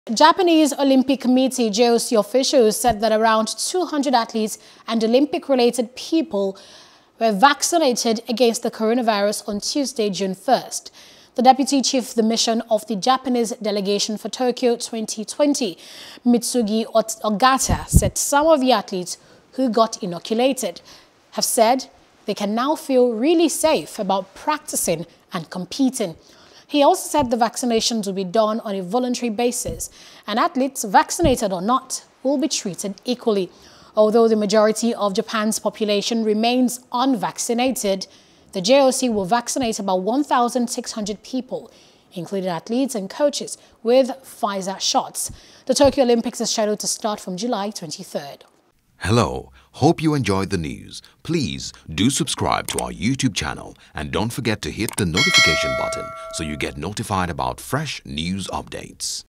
JOC: About 200 Japanese Athletes And Olympic Related People Vaccinated | SPORTS
Japanese Olympic Committee (JOC) officials said that around 200 athletes and Olympic-related people were vaccinated against the coronavirus on Tuesday (June 1st.